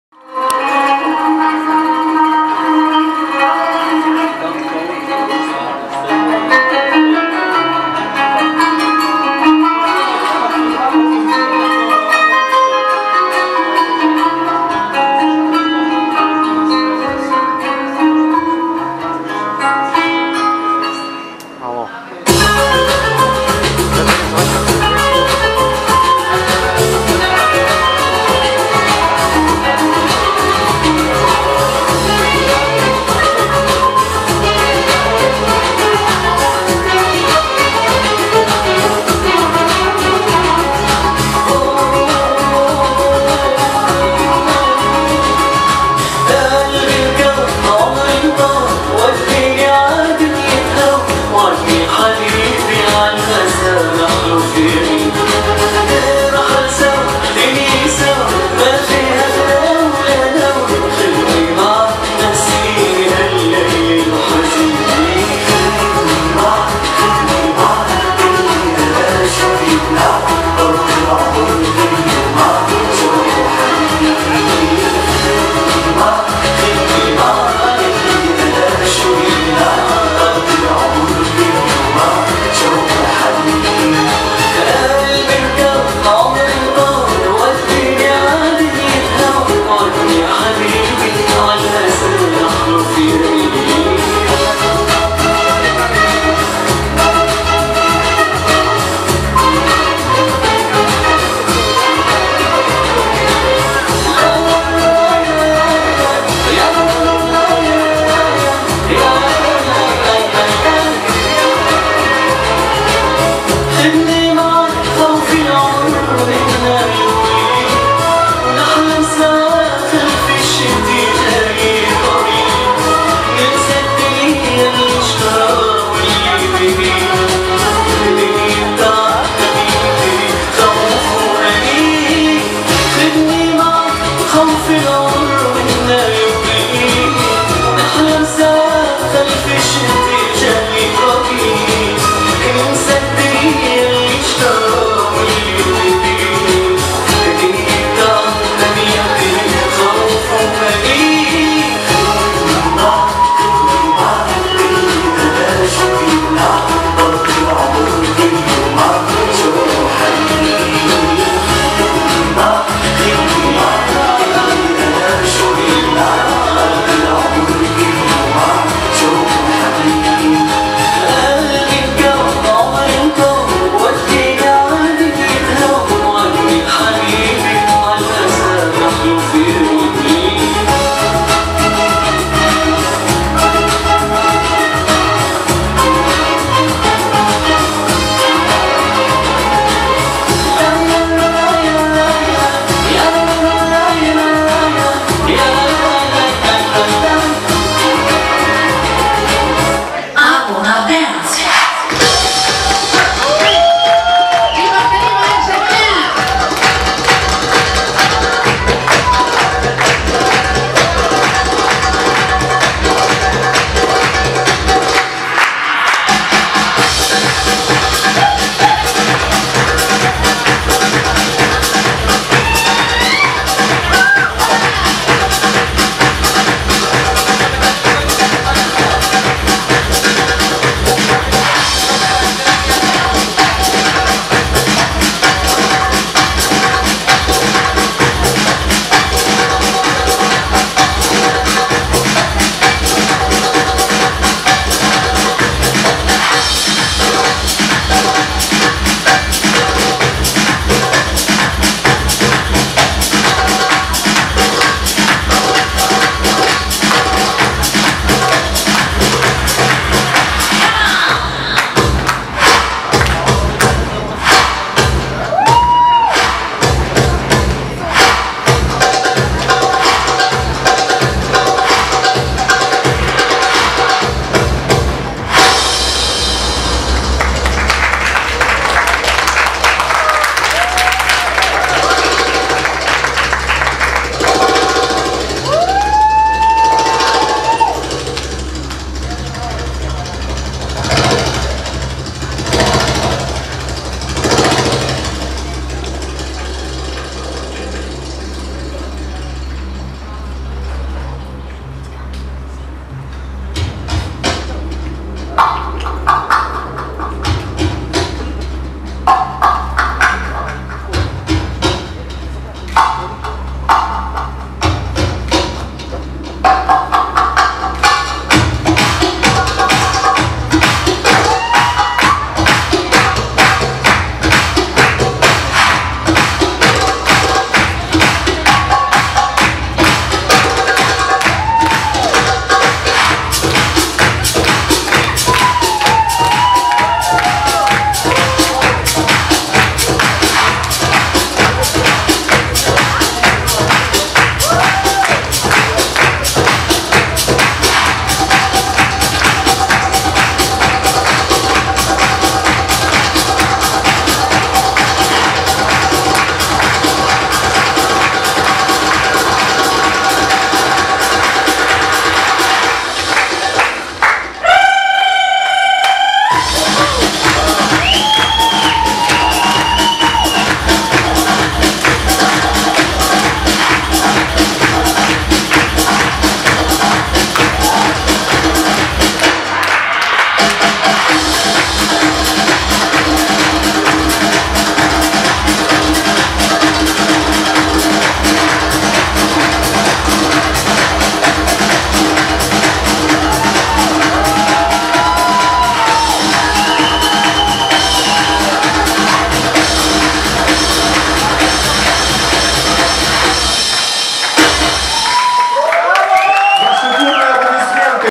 "Crystal Belly Dance" 30.08.2015
30 августа 2015 в г.Симферополь состоялось торжественное открытие танцевальной студии Абрахим Ханан "Belly Dance Qween"
Ученицы танцеввльной студии "Crystal Belly Dance" г.Севастополь и руководитель студии Наталья Ваганова поздравили Ханан танцевальным шоу номером !